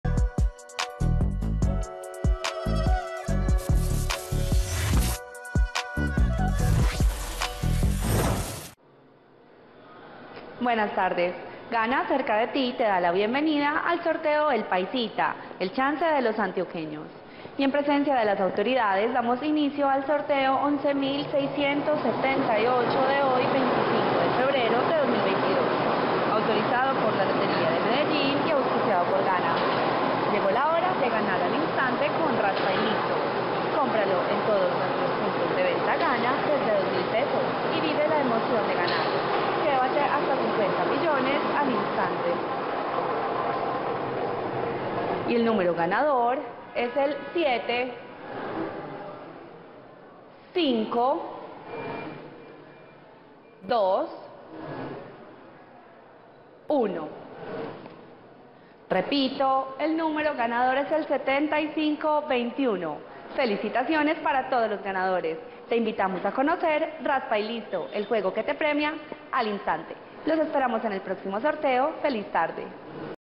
EL PAISITA 2 Resultados del Chance del VIERNES 25 de febrero de 2022 ✅🥇🔥💰
Hola buenas noches, te presentamos los resultados de las loterías y chances en Colombia, vamos con el Resultados del Chance El Paisita 2 del VIERNES 25 de febrero de 2022.